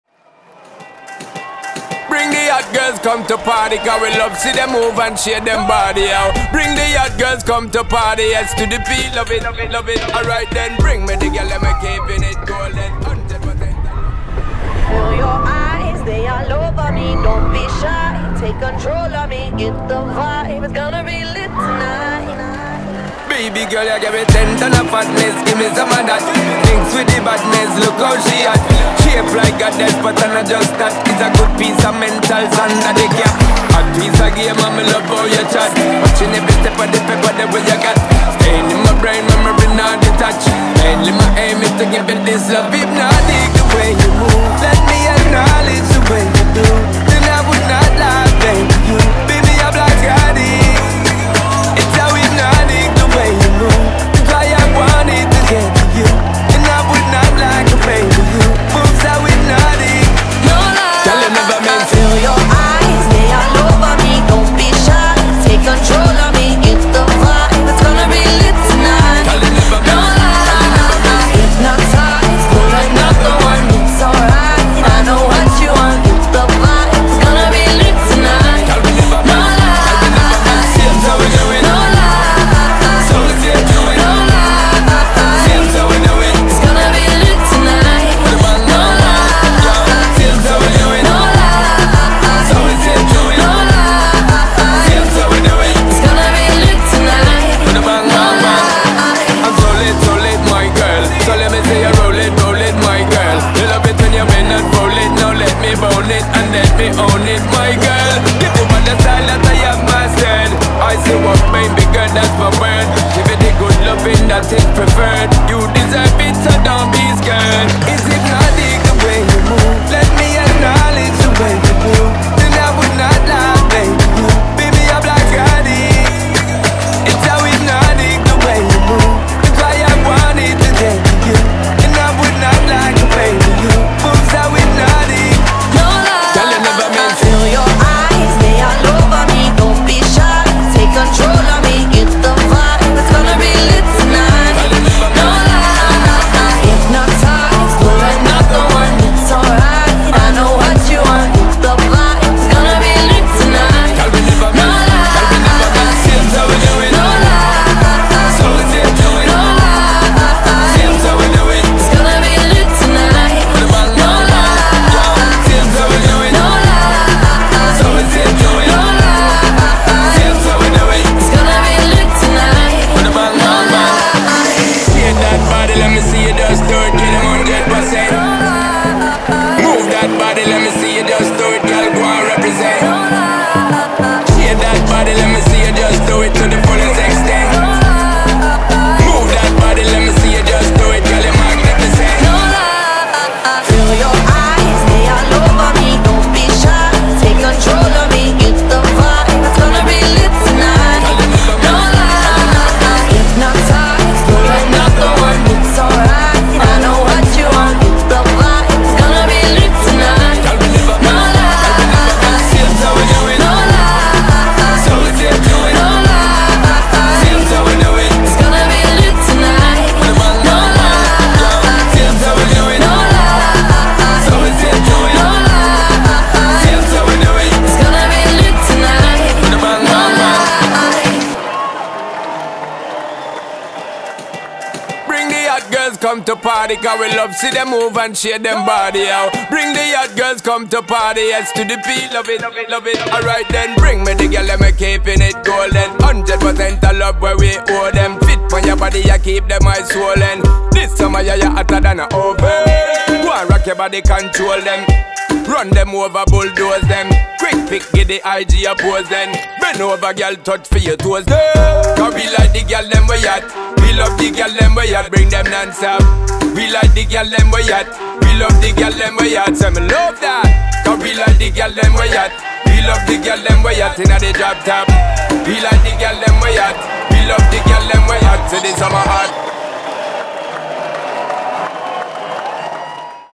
Sean Paul a Roma - LUXURY EVENTS | UNDERMOON
Il video del concerto di Sean Paul a Cinecittà World dell'8 settembre 2023
Unica tappa italiana dello Sean Paul World Tour 2023
LUXURY EVENTS | UNDERMOON